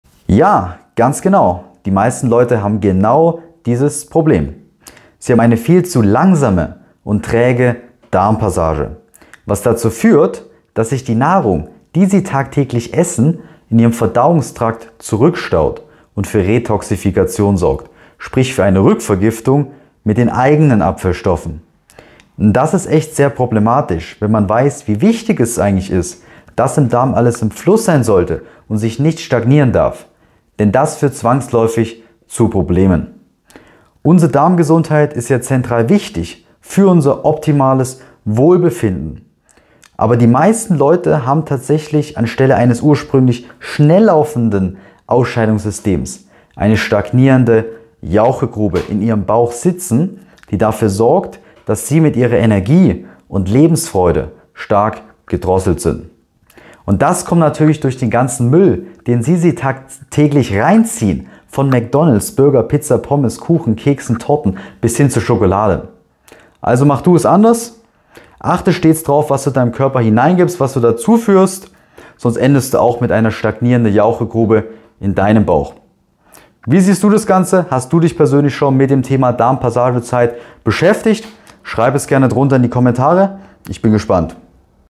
‼️ Die meisten Menschen haben genau dieses Problem…
Schau dir das Video in voller Länge an, um den Sachverhalt zu verstehen.

Hast du dich schon mal mit dem Thema Darmpassagezeit beschäftigt?
Schreib es gerne drunter in die Kommentare 👇